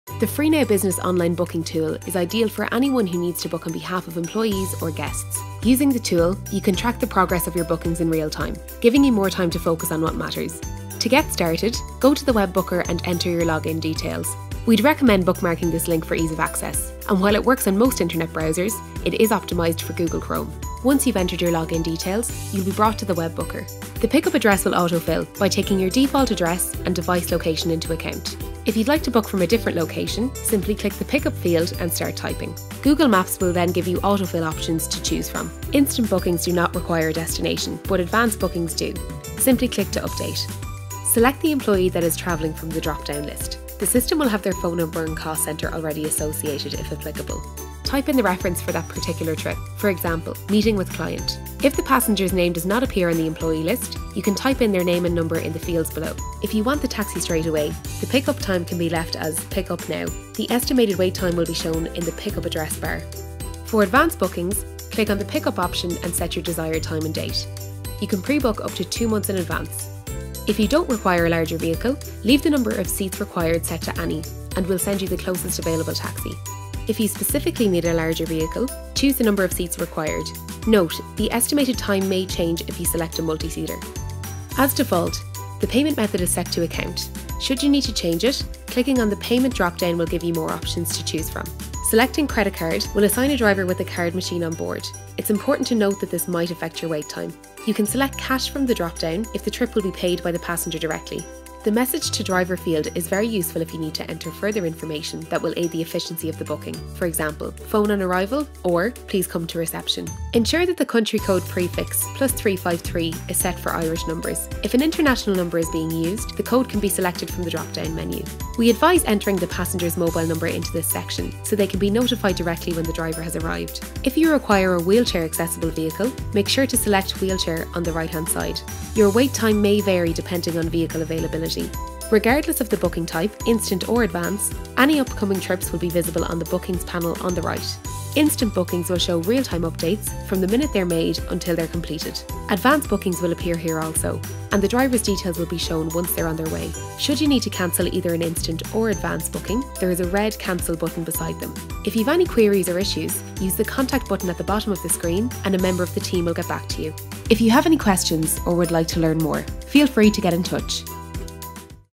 How to use the FREE NOW Web Booker (corporate)
The FREE NOW Web booker is a powerful tool that gives you fill visibility over your business travel. Here is a short tutorial on how to use the Administration Panel on your FREE NOW Business account.